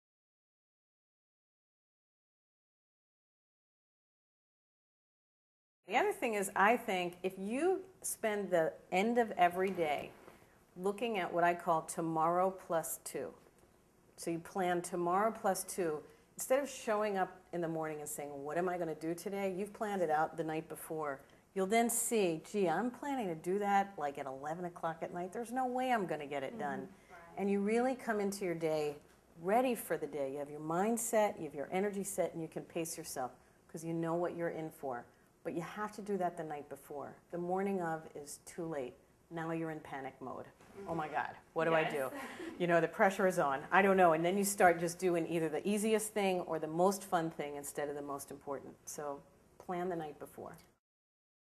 Julie Morgenstern Advice On Planning Ahead
Watch OneLessStress Guest Expert Julie Morgenstern Give Advice On The Best Ways To Improve Your Time Management Skills and Plan Ahead